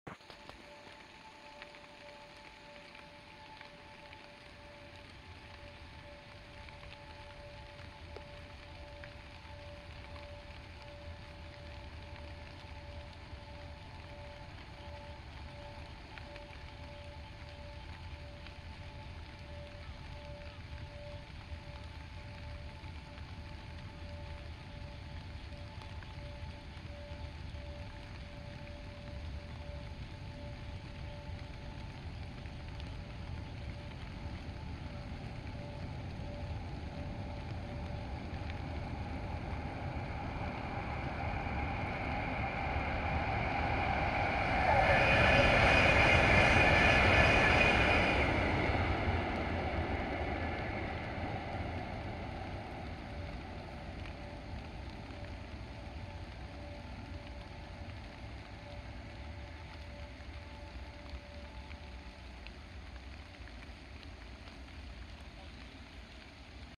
お疲れ様MAXとき号 新潟行 通過シーン
JR東日本
JR上越新幹線

先日定期運行の終了したE4系MAX新幹線
見られるのもあと僅かですね。